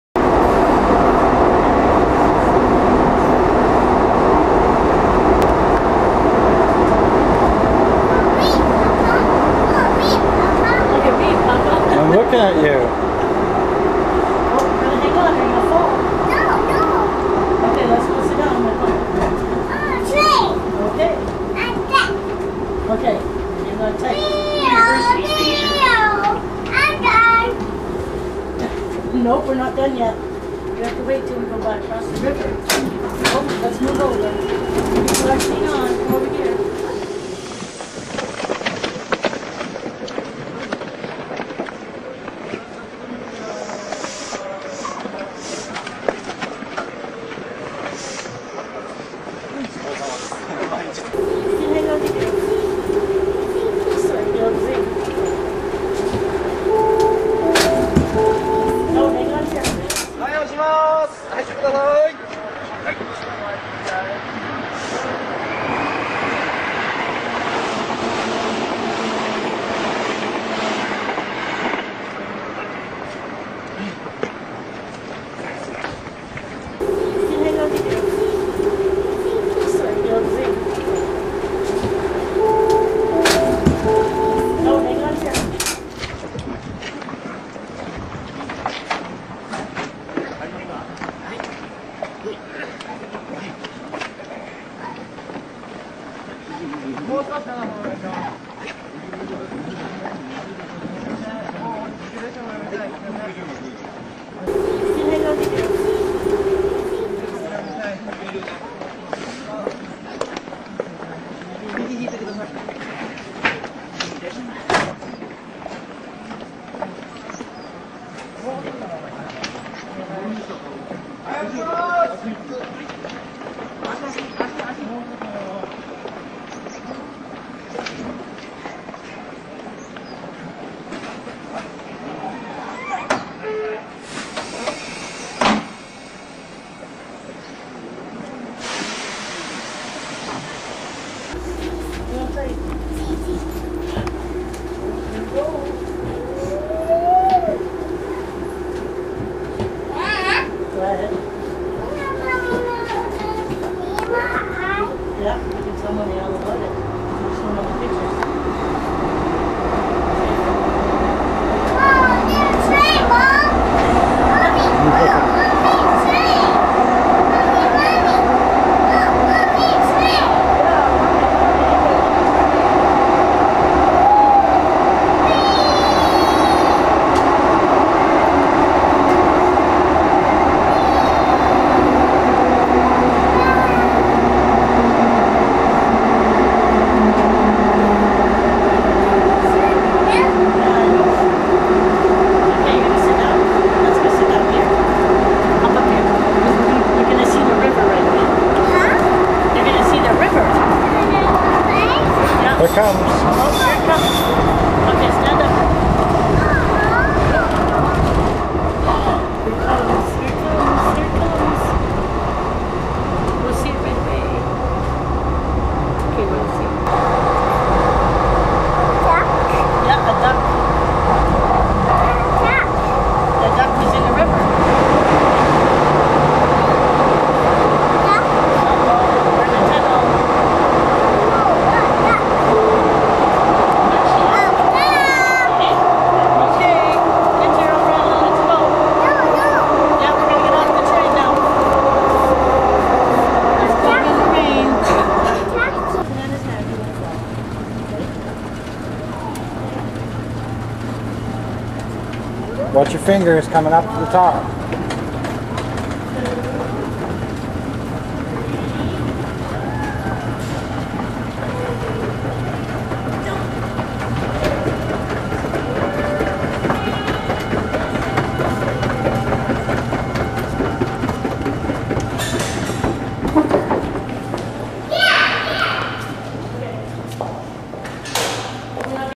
train train part 2
We take our grand daughter on the LRT (Light Rail Transit) but tell her she's on a train